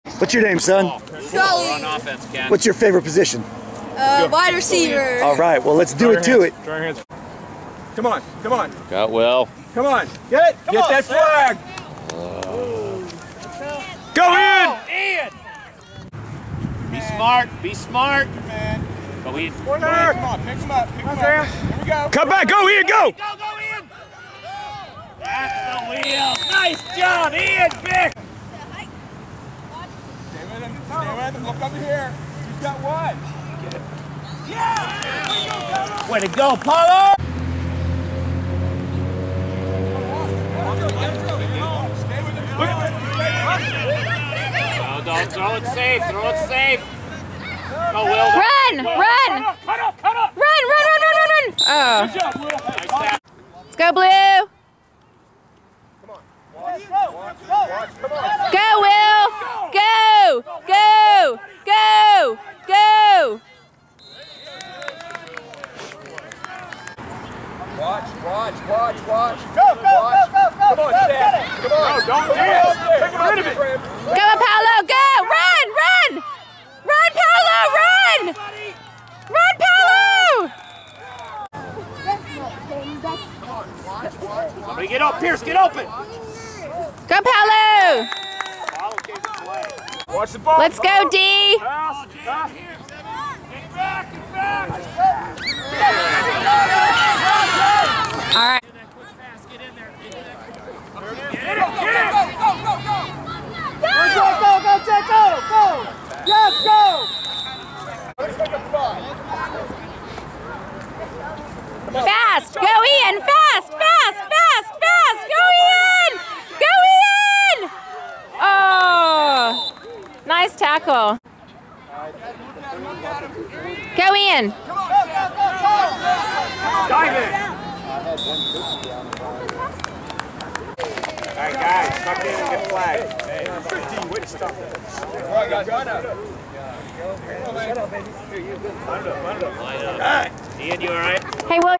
2015-01-11 - St. George vs Bills - 4th grade flag football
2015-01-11
St. George vs Bills
Won 13-0
4th grade flag football